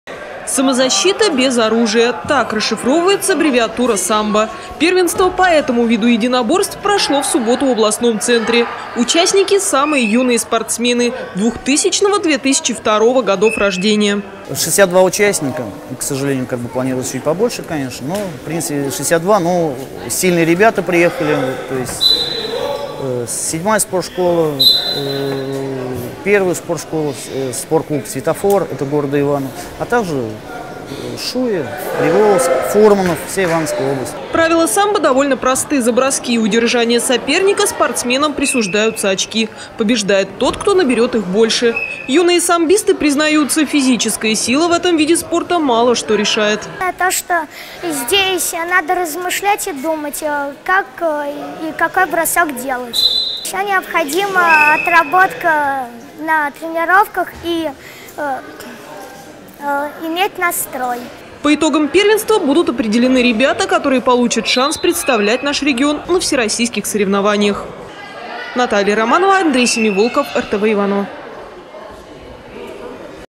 Городское первенство по самбо
Самозащита без оружия. Так расшифровывается аббревиатура самбо. Первенство по этому виду единоборств прошло в субботу в областном центре.